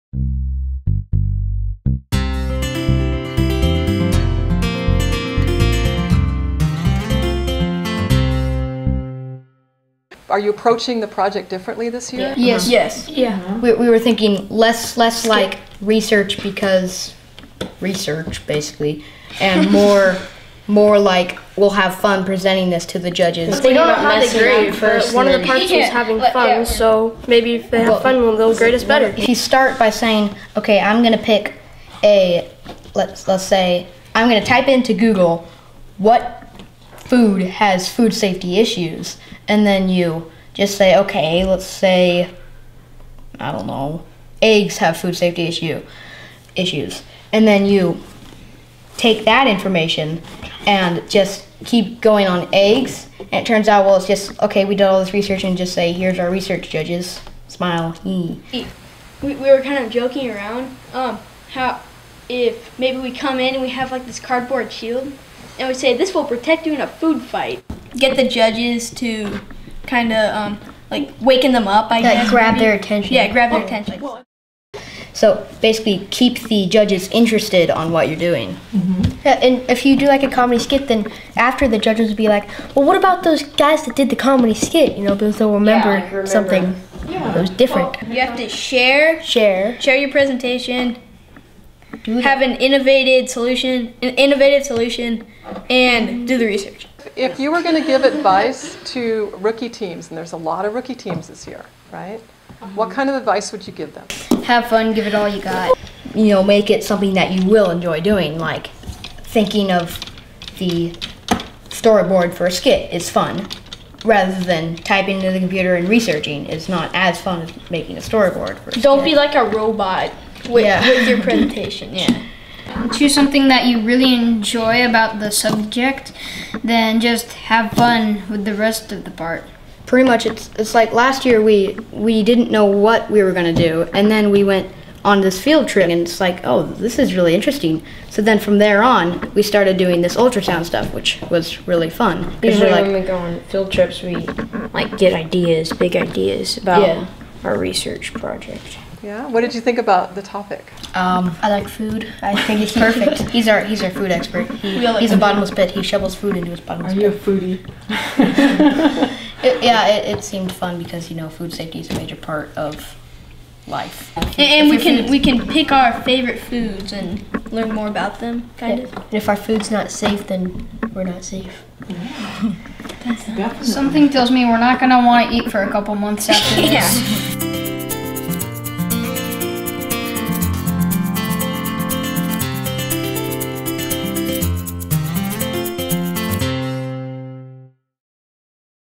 Project Avenue - Food Factor - Episode 4
FIRST LEGO League - Project Avenue

-- Episode 4: Making the project fun --

Team BEBO shares some thoughts and advice for rookie teams.